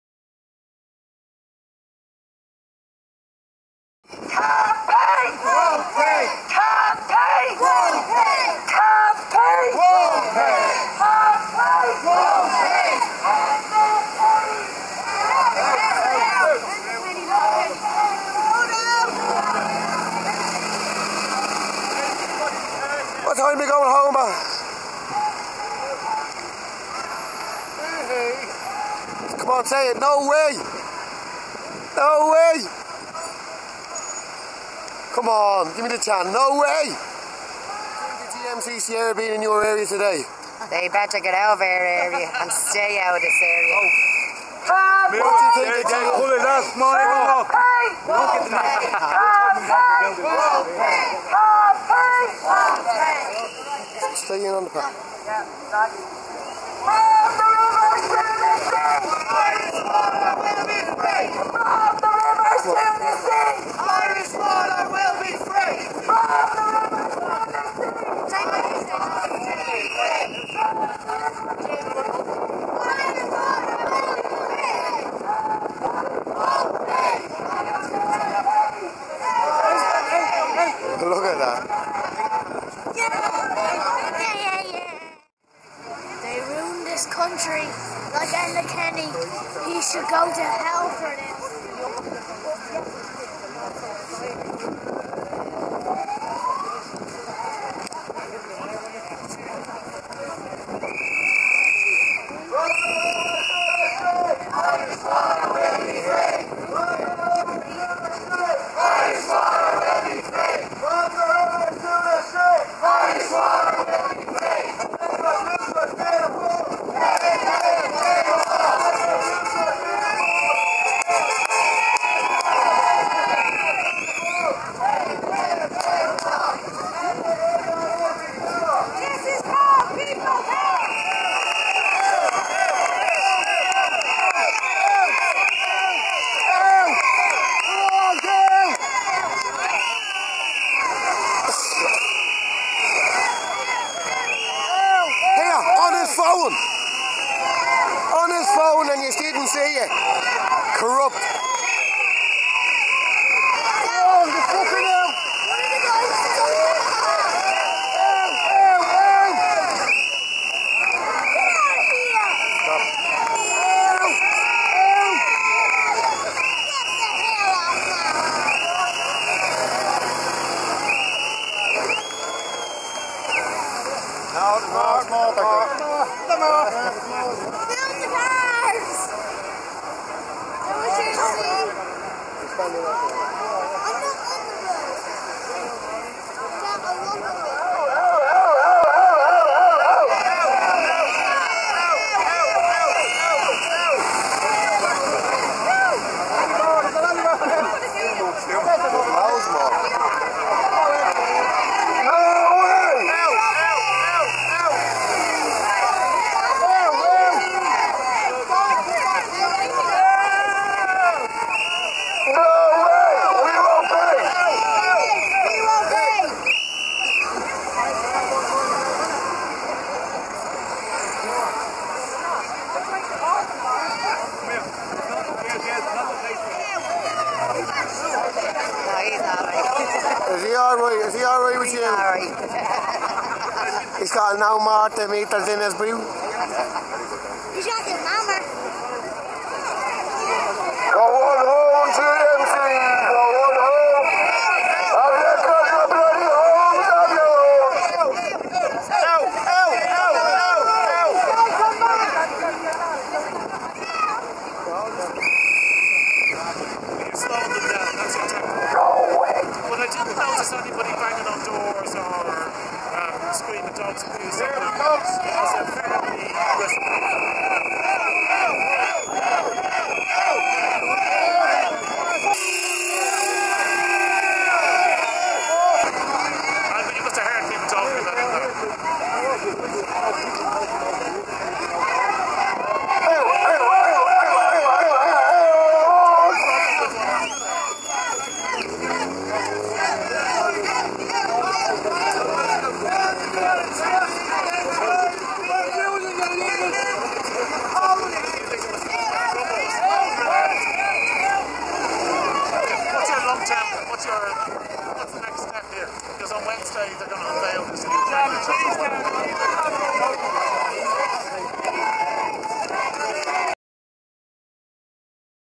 GMC SIERRA (The Long Goodbye)
The convoy of GMC SIERRA AND MURPHYS CONTRACTORS SLOWLY MARCHED OUT OF DONAGHMEDE TODAY